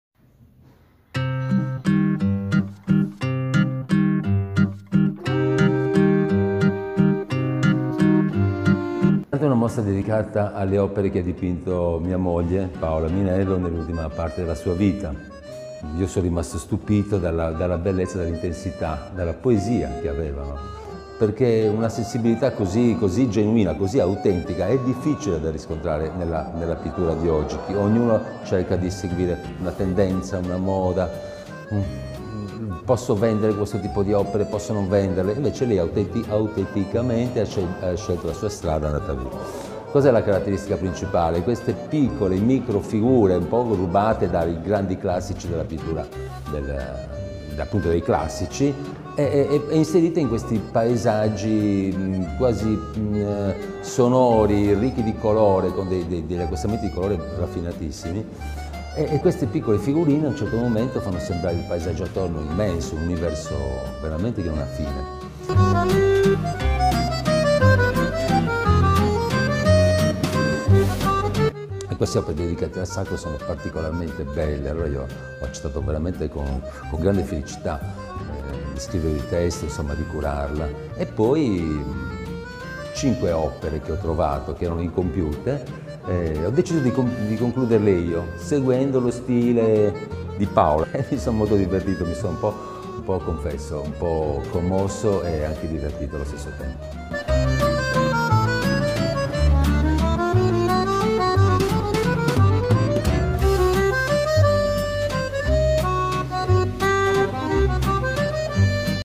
Paola Minello, quando la sacralità entra nell'immenso. Interessante mostra al Museo Diocesano
"L'immenso minuscolo" è l'esposizione dedicata alla moglie dell'artista Salvatore Garau. Oggi l'inaugurazione